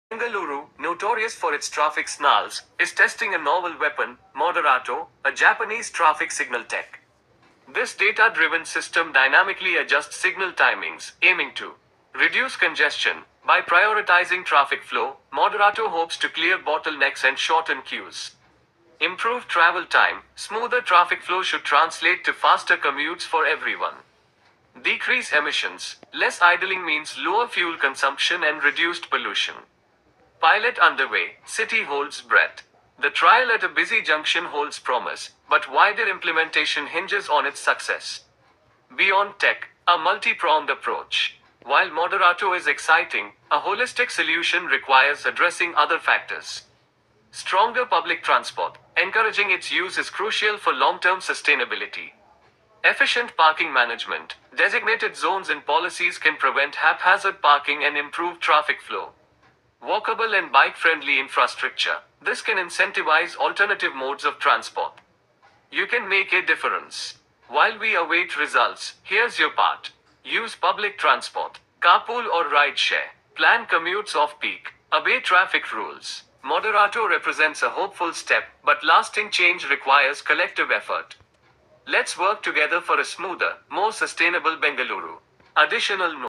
🚨 Bengaluru trials Japanese traffic signal technology to reduce congestion JAPS BEST?
Video from Salesachin

BIGOBASKET UNIVERSITY SALES SALESACHIN

Bengaluru's Traffic Woes Get a Tech Makeover: Can MODERATO Tame the Beast?

Bengaluru, notorious for its traffic snarls, is testing a novel weapon: **MODERATO, a Japanese traffic signal tech**. This data-driven system dynamically adjusts signal timings, aiming to:

Reduce congestion: By prioritizing traffic flow, MODERATO hopes to clear bottlenecks and shorten queues.
Improve travel time: Smoother traffic flow should translate to faster commutes for everyone.
Decrease emissions: Less idling means lower fuel consumption and reduced pollution.

Pilot Underway, City Holds Breath

The trial at a busy junction holds promise, but wider implementation hinges on its success.

Beyond Tech: A Multi-Pronged Approach:

While MODERATO is exciting, a holistic solution requires addressing other factors:

Stronger public transport Encouraging its use is crucial for long-term sustainability.
Efficient parking management Designated zones and policies can prevent haphazard parking and improve traffic flow.
Walkable and bike-friendly infrastructure:This can incentivize alternative modes of transport.

While we await results, here's your part:

Use public transport
Carpool or rideshare
Plan commutes off-peak
Obey traffic rules.

MODERATO represents a hopeful step, but lasting change requires collective effort. Let's work together for a smoother, more sustainable Bengaluru Karnataka